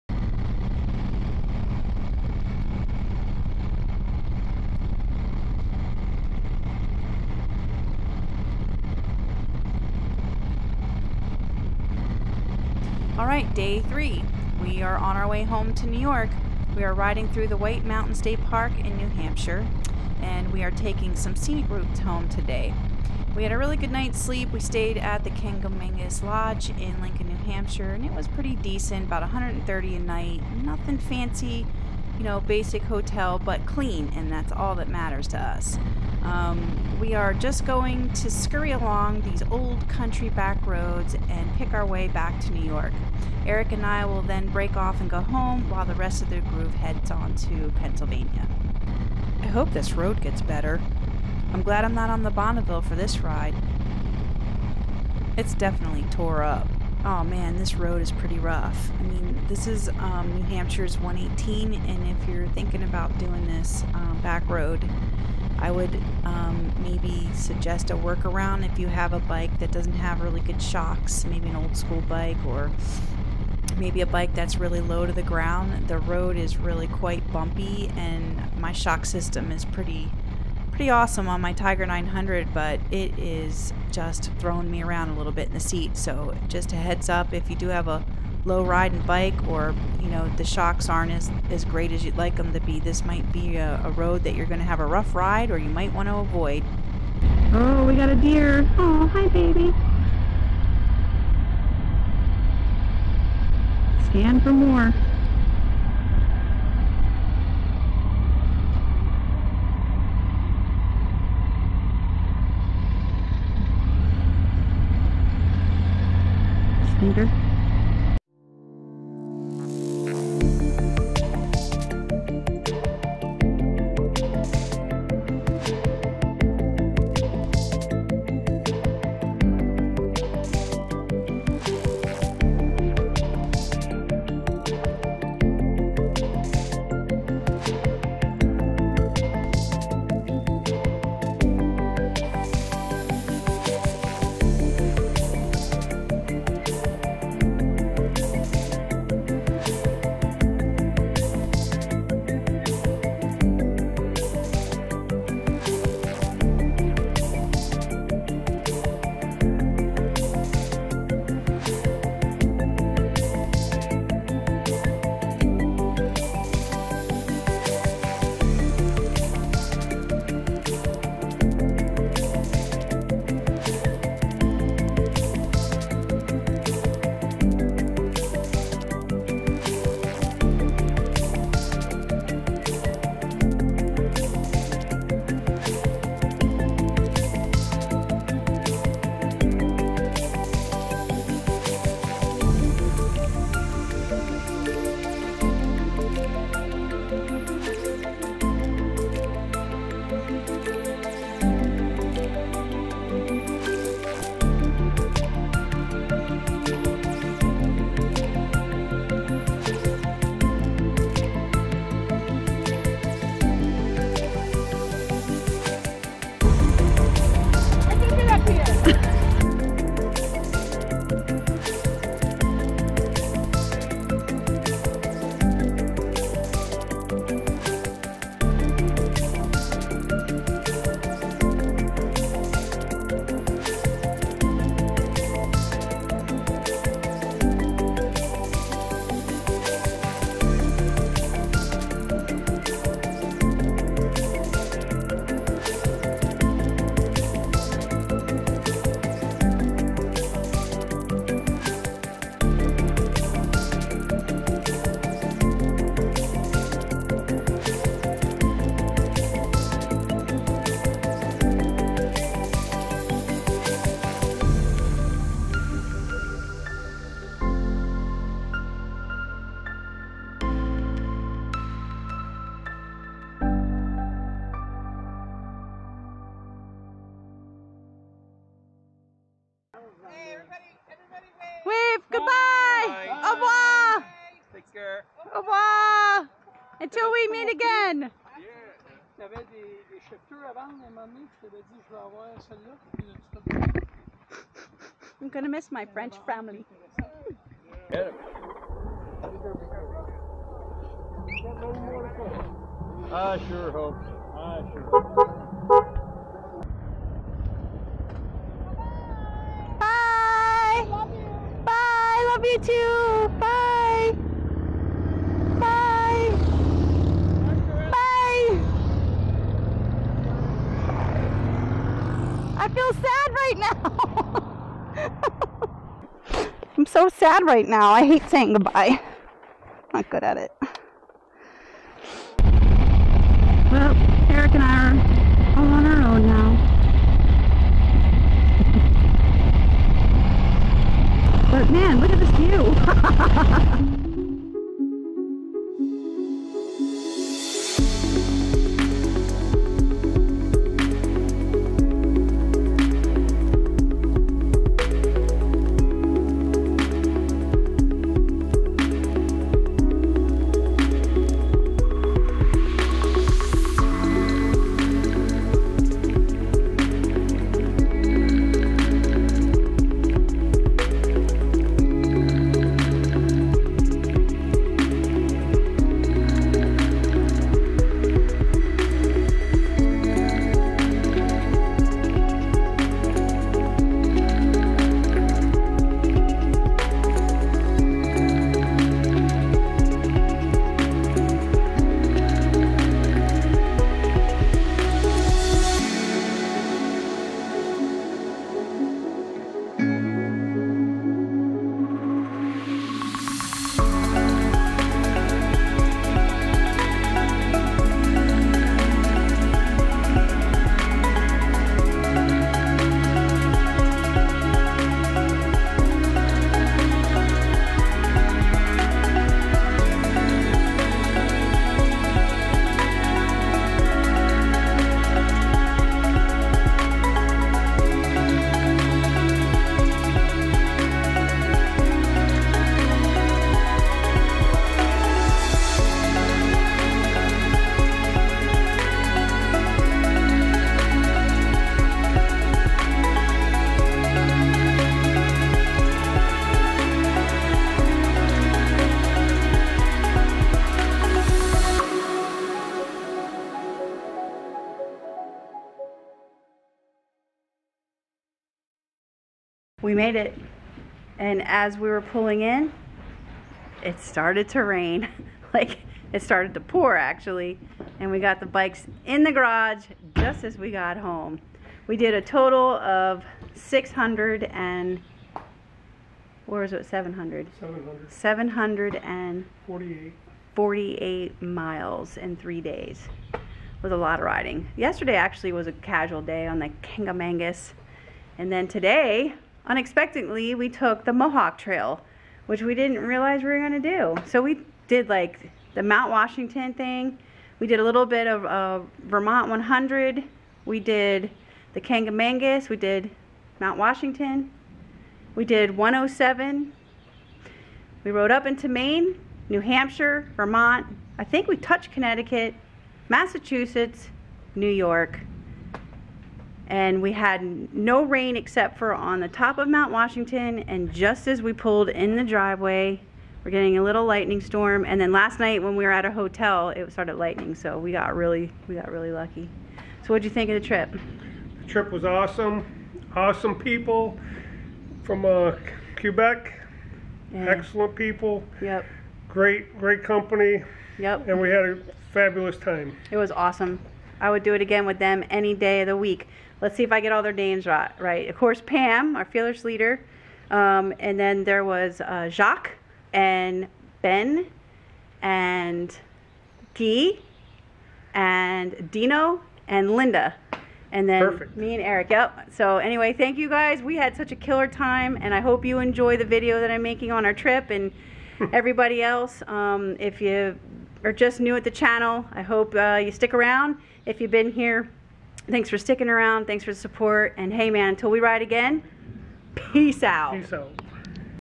NEW ENGLAND DAY 3
This was the last day of our New England trip with our Blue Knight family from up north.  We were not ready for it to end. I love riding through the New England states and seeing all the historic towns and villages.  The views, rivers and farms were beautiful.  There is nothing like a vacation on two wheels and nothing better than spending it in good company.